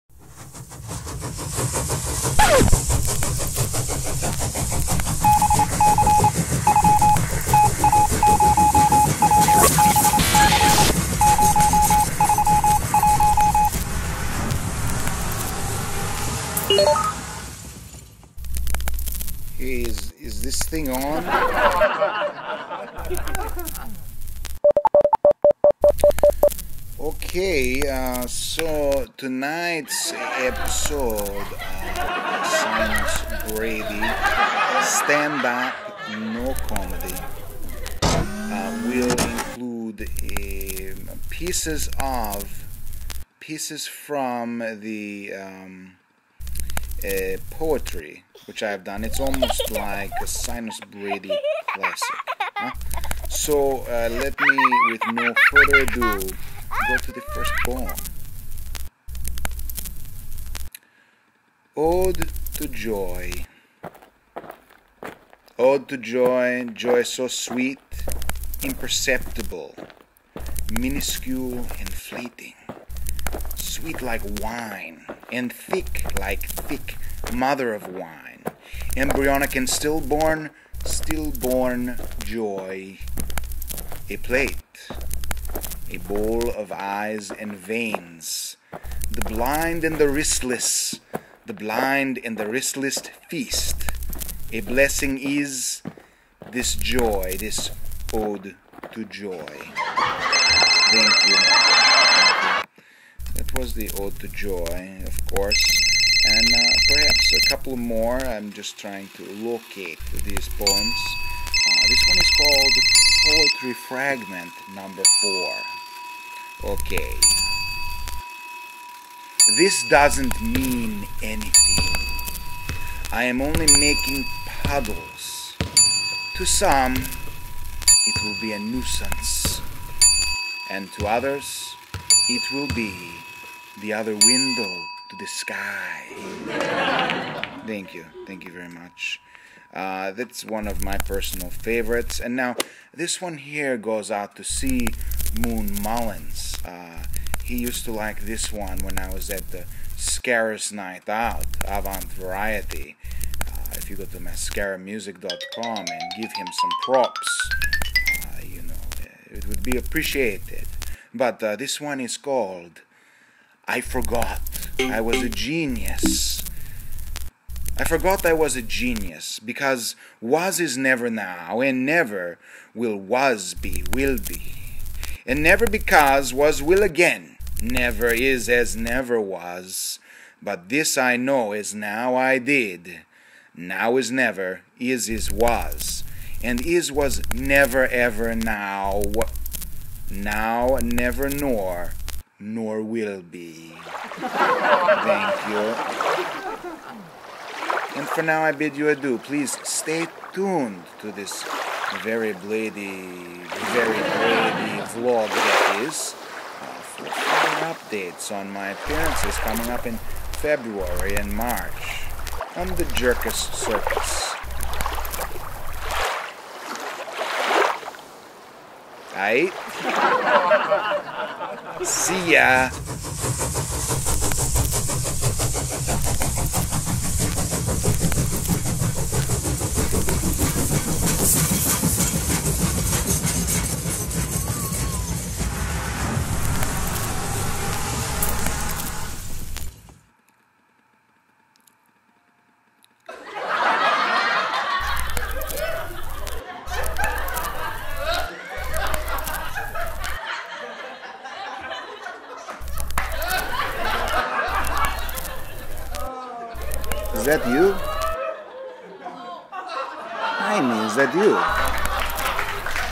a few poeMs for you, no?
SiNuS BRaDy presents even more of the non-stop hilarity of his 'Stand Up | no comedy' act ... right here ... on the YouTube. There is no other pre-recorded, live act out there that seemingly stays a bit below the funny mark, always hitting the strange zone. Enjoy.